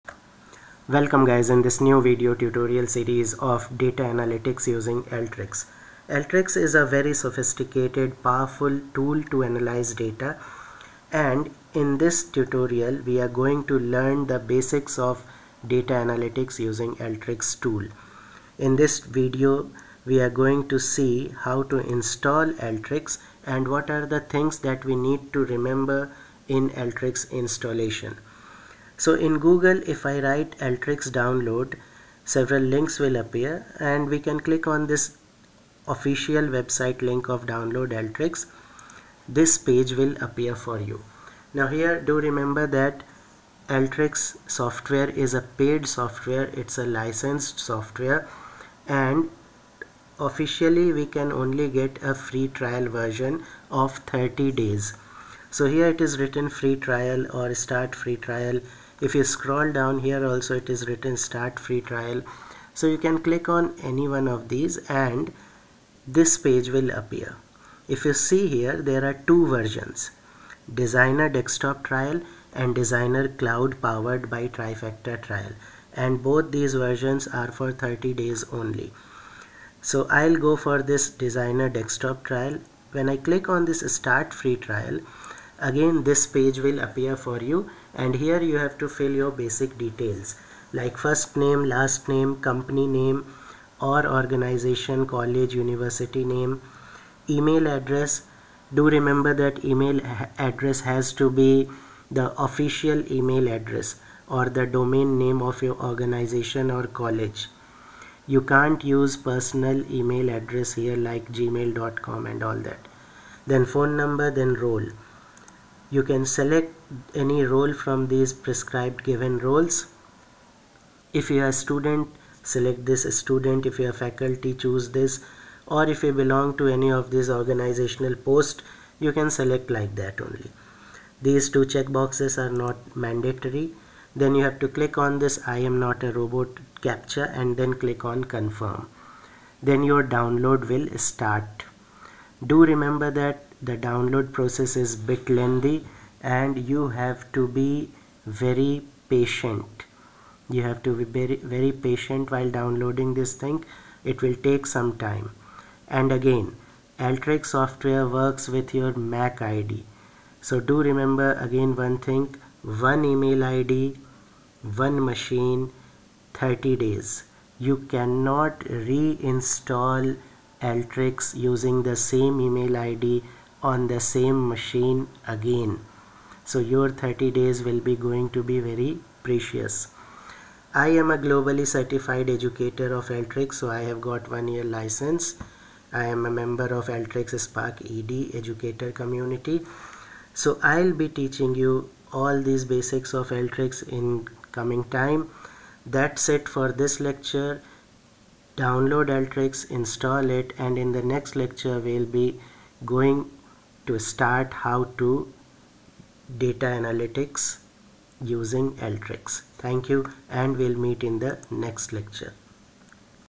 #1. Installing Alteryx Designer
In this lecture we'll learn how to install Alteryx Designer Software.